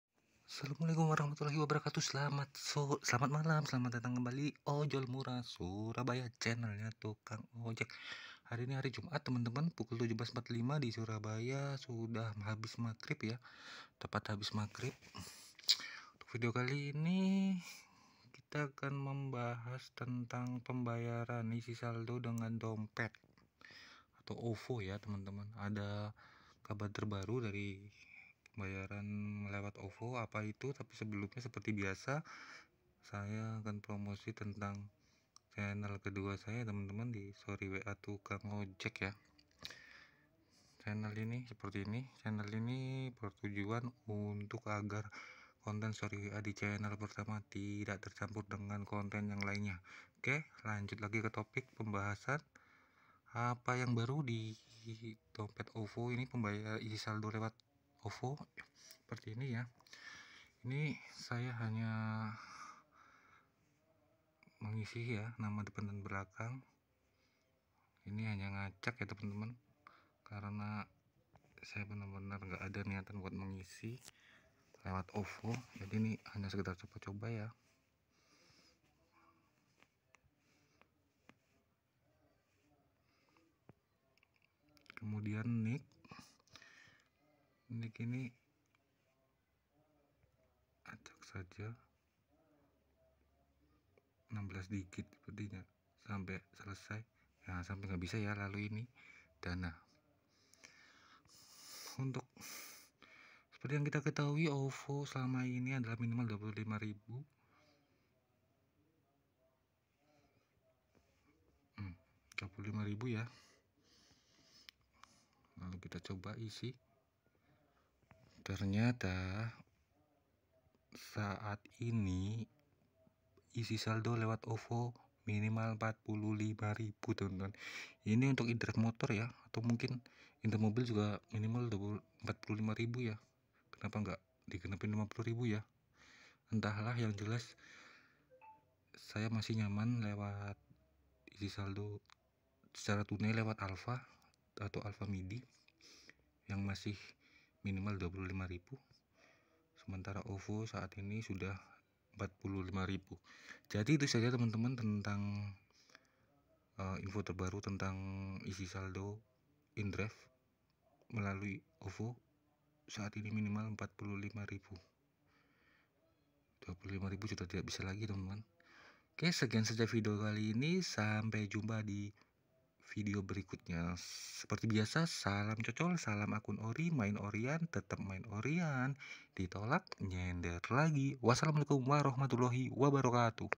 ISI SALDO INDRIVE LEWAT OVO SEKARANG GAK BISA 25K
Top up saldo indrive lewat dompet digital ovo sekarang minimal gak bisa 25ribu lagi